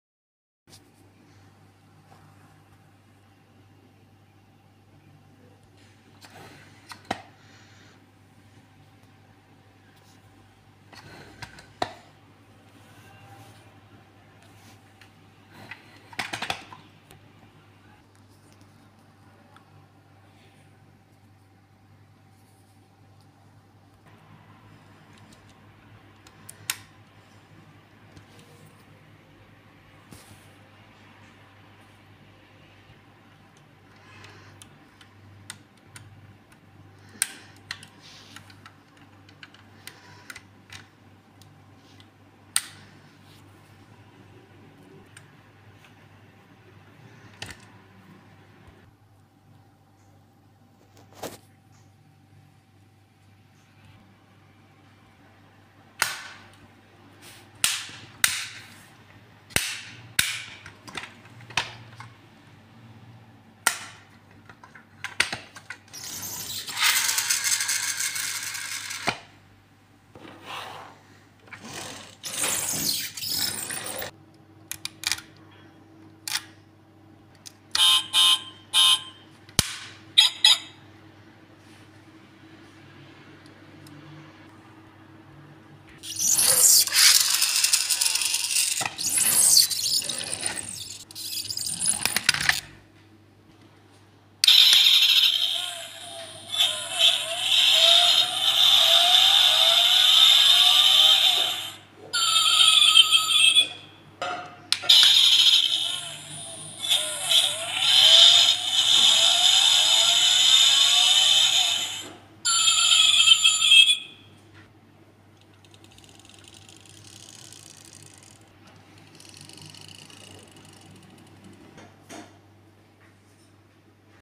review of diecast Toyota land l300 😮😲🧐toyota
diecast model car

1:32 scale model car

Toyota land cruiser l300

copy of real land cruiser

color is white

paint coulity is very good

price is 3000

bought this car from daraz